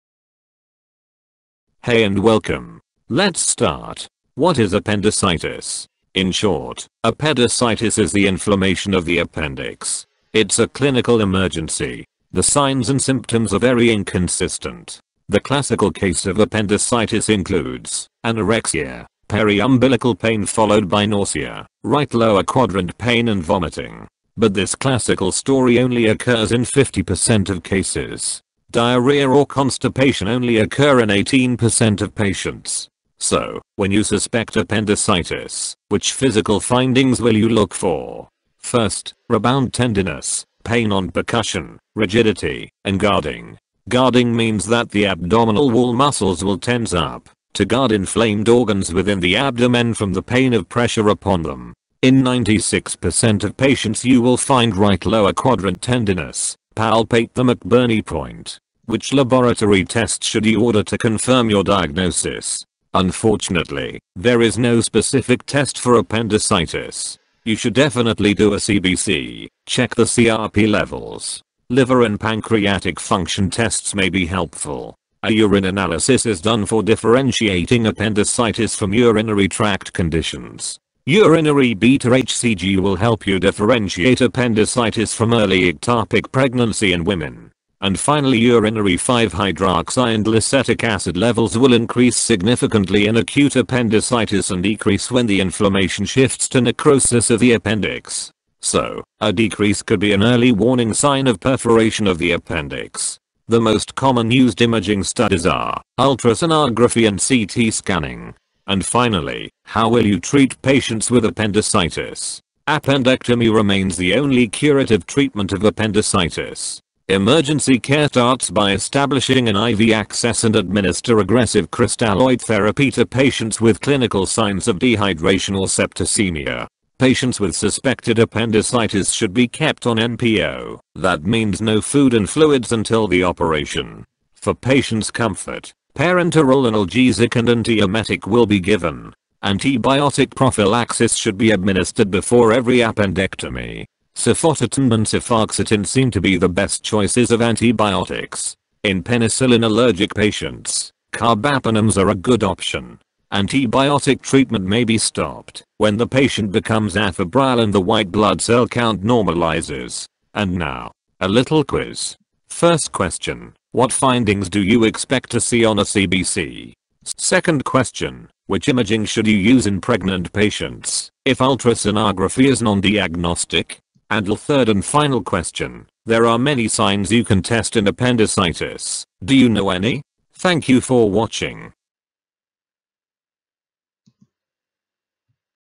appendicitis: short presentation
What is appendicitis? What are the symptoms? How do you make a diagnosis? What is the treatement? In this video you will get an answer to all these questions!

All suggestions and tips are welcome!
Hope you enoy the video!